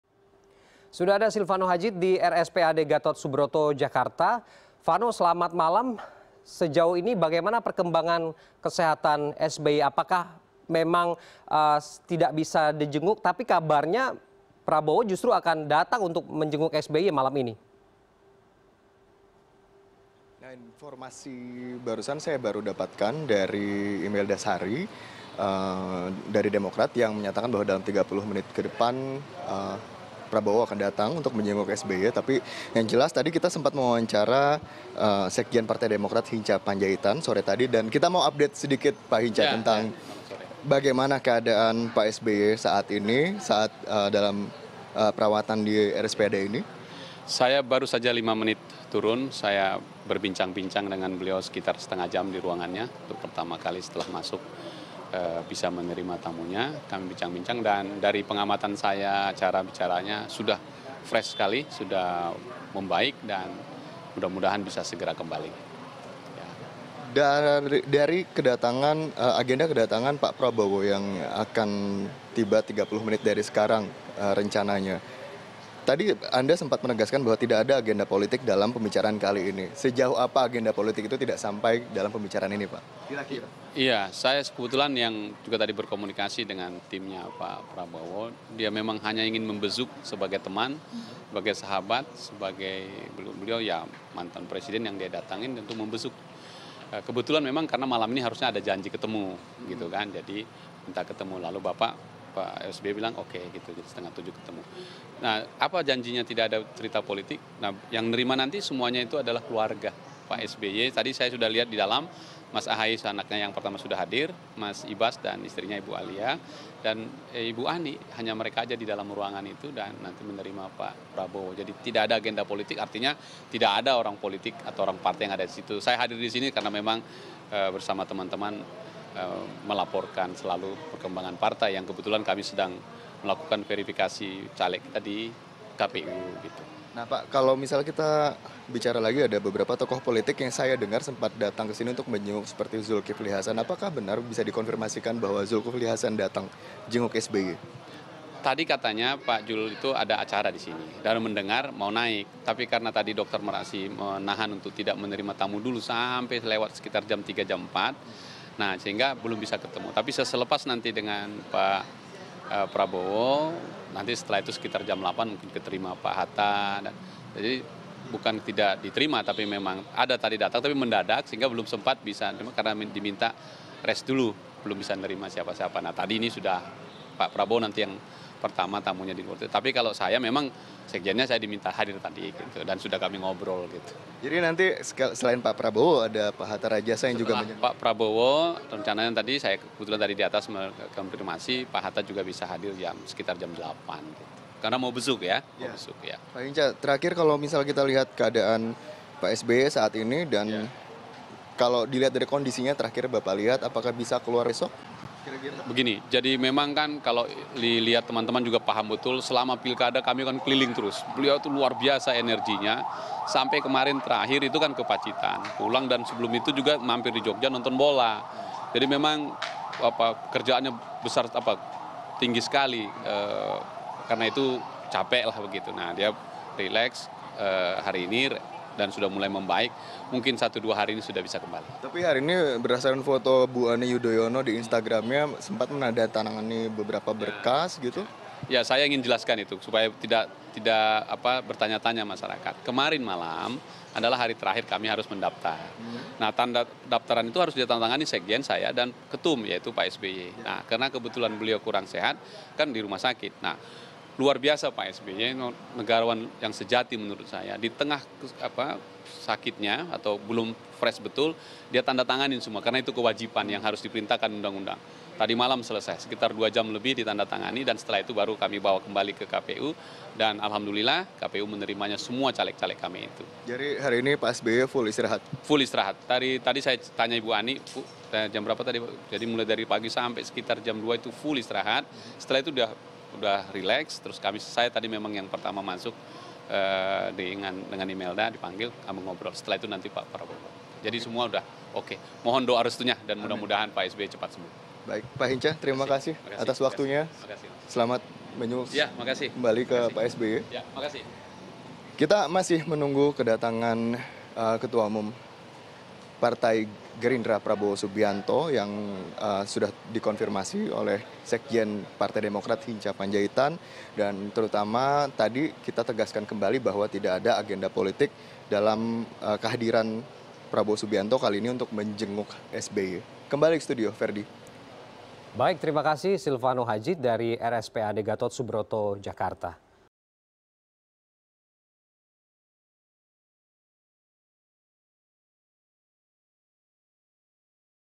Hinca Panjaitan: Prabowo Akan Jenguk SBY
Sekjen Partai Demokrat, Hinca Panjaitan memastikan, Ketua Umum Partai Gerindra, Prabowo Subianto akan menjenguk SBY sore ini di RSPAD

Like & Follow Facebook:
CNN Indonesia

cnnindonesia